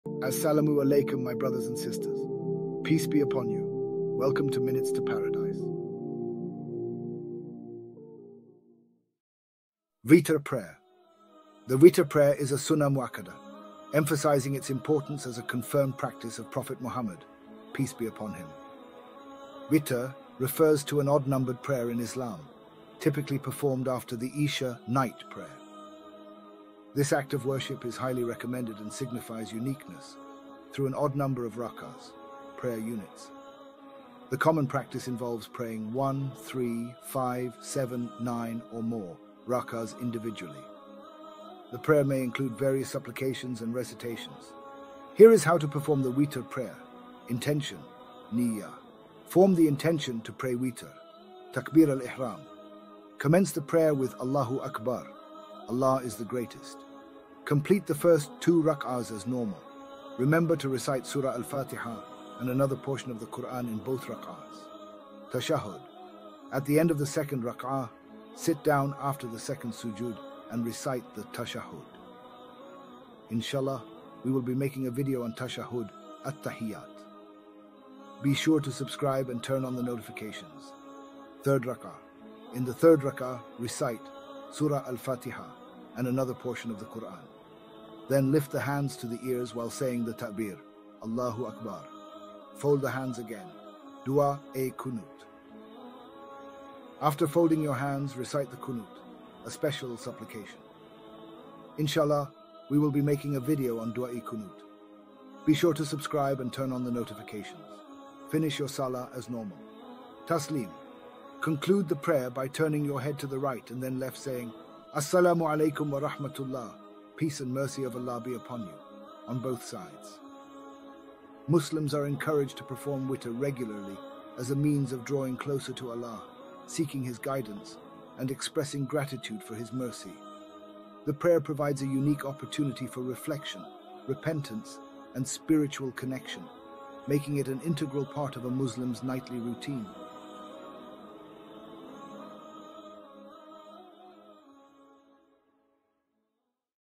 Witr Prayer
Explore the profound significance of the Witr prayer in Islam, a unique act of worship performed after Isha, designed to bring Muslims closer to Allah. Discover the flexibility in performing this prayer, with options for one, three, five, seven, or more rak'ahs. Join us as we guide you through the steps, from the Niyyah (intention) to the Qunoot supplication, providing a spiritual journey for reflection, repentance, and connection with Allah. Embrace the richness of Islamic tradition with our insights into this Sunnah Mu'akkadah, recommended by Prophet Muhammad (peace be upon him).